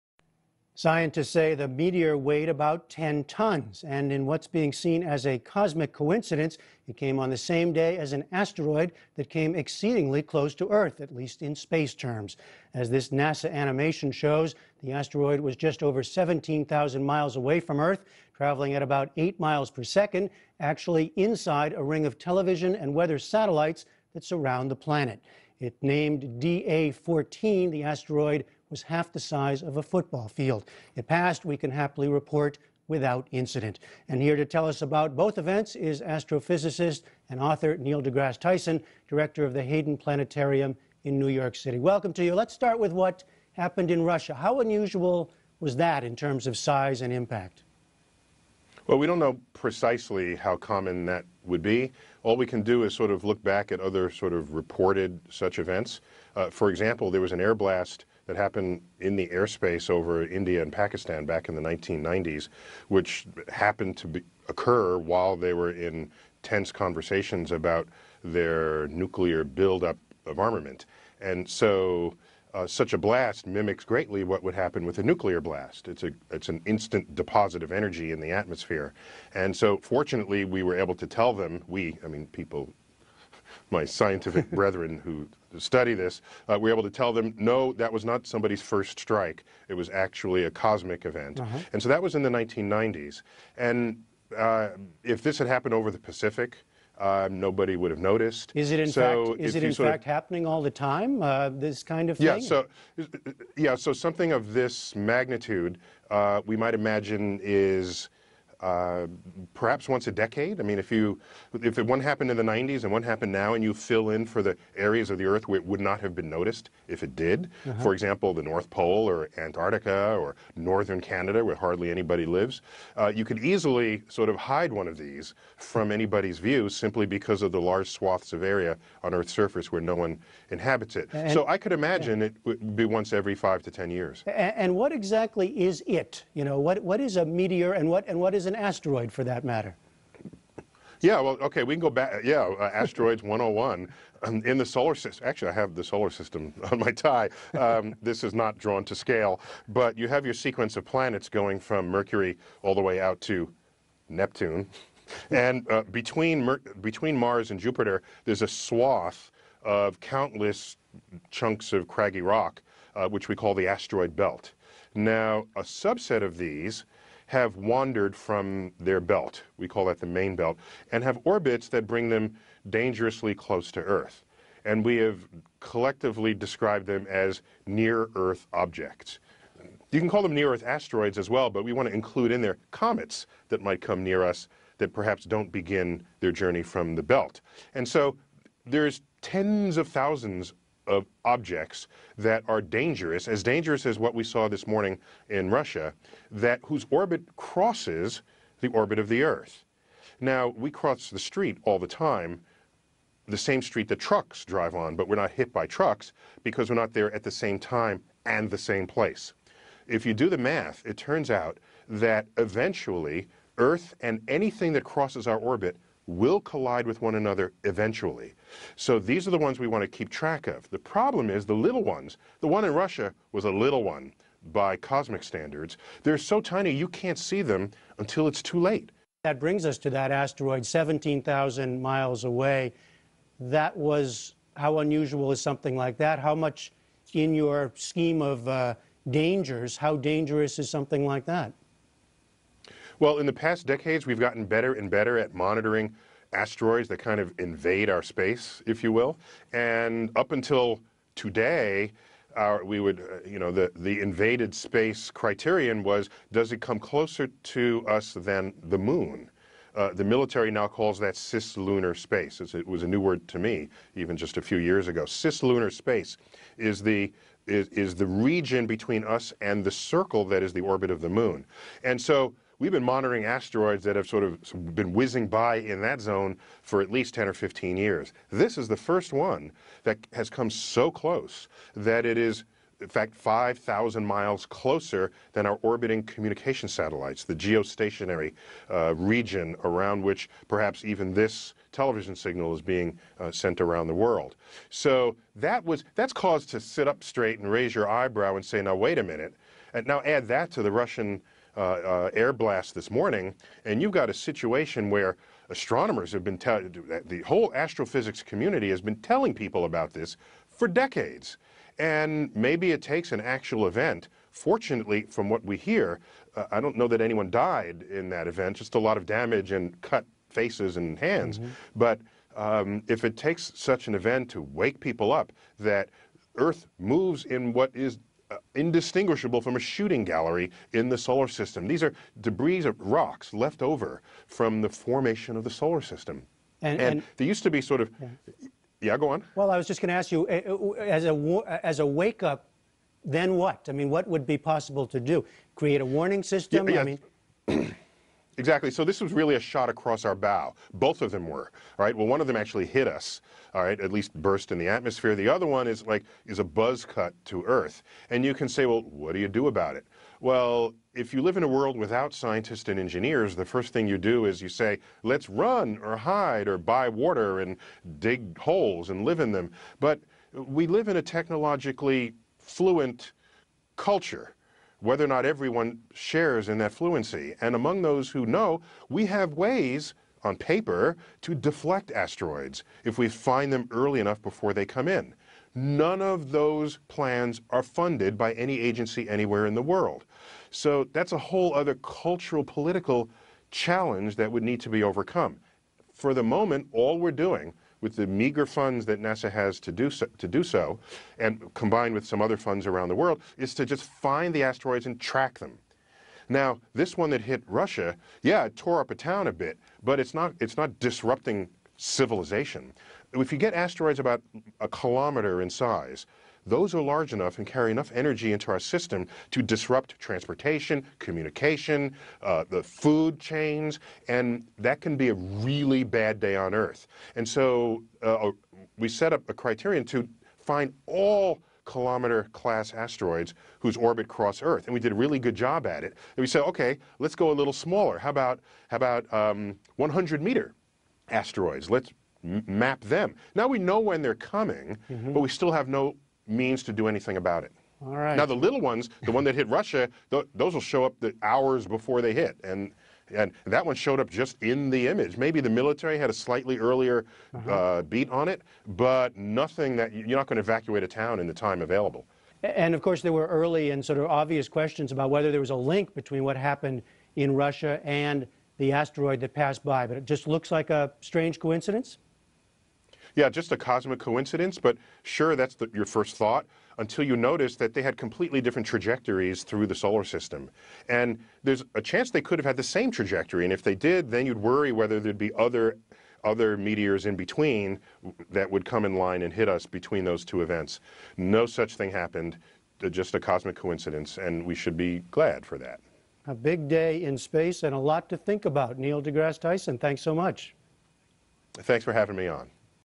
Neil deGrasse Tyson A Passagem do Asteróide e o Meteoro na Rússia
Vídeo legendado por: Luc Anderssen